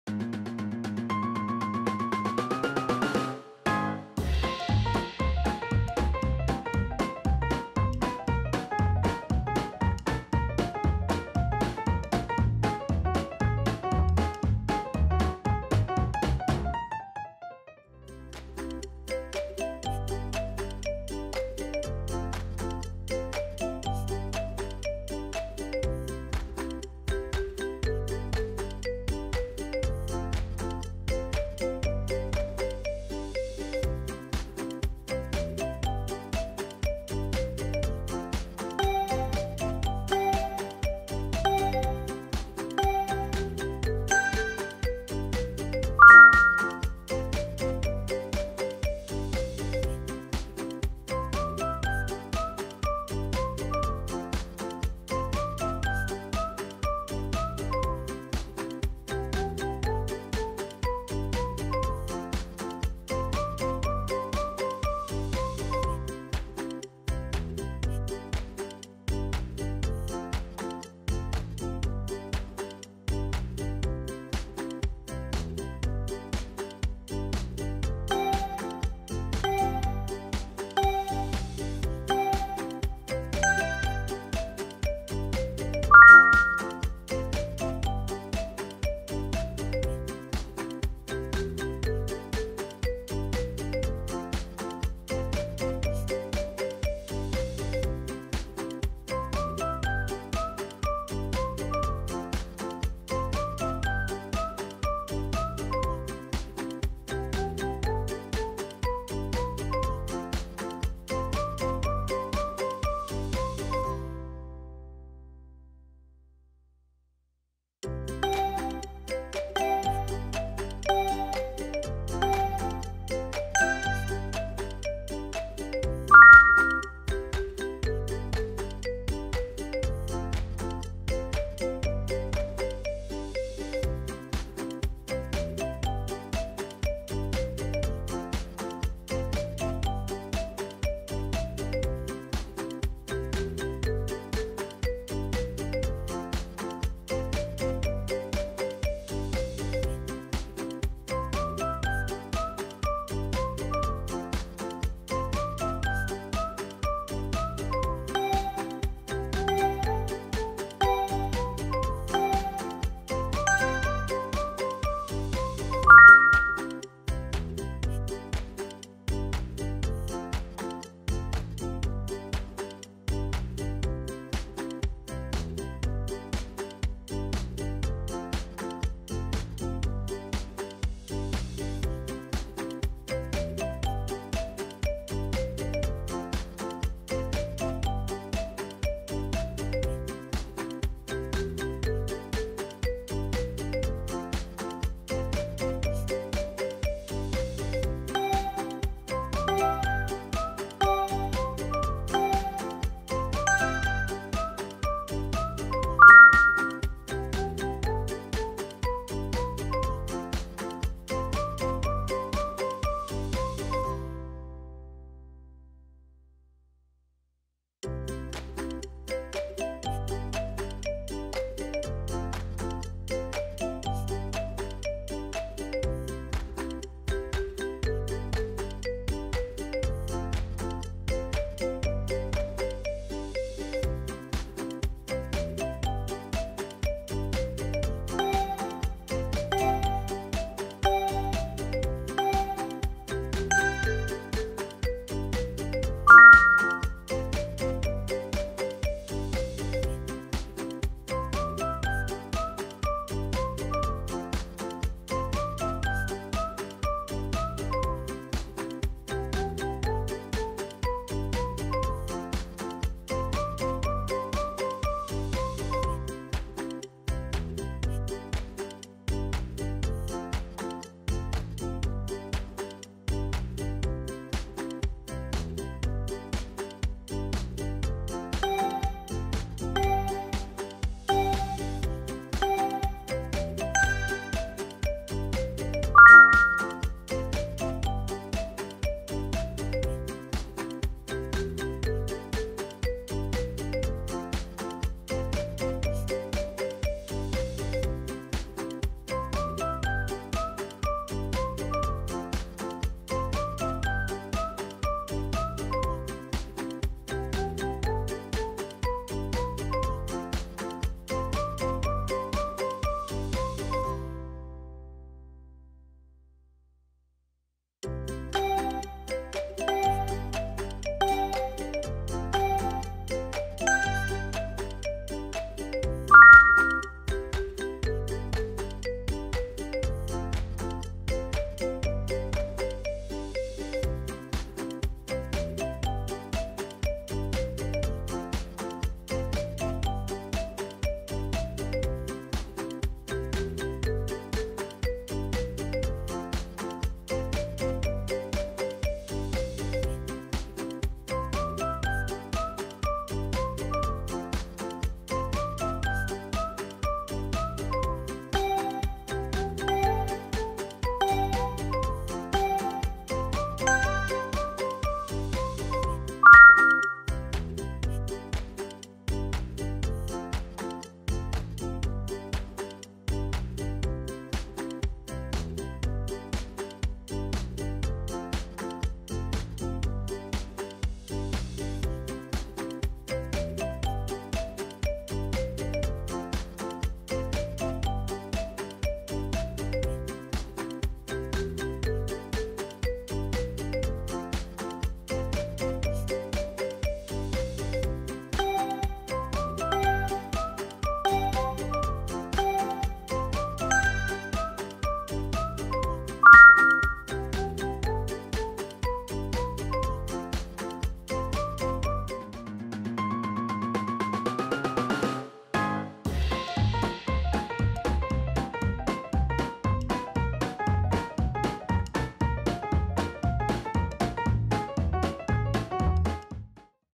Can you Spot the Differences in these Disney puzzles?
Disney Spot the Difference Puzzle Game! We have 10 rounds of Disney Spot the Difference Puzzles and in each round you will have to find 3 differences. In each round of this brain puzzles game one of our Disney movie favourites is displayed and you have 25 seconds to solve the puzzle!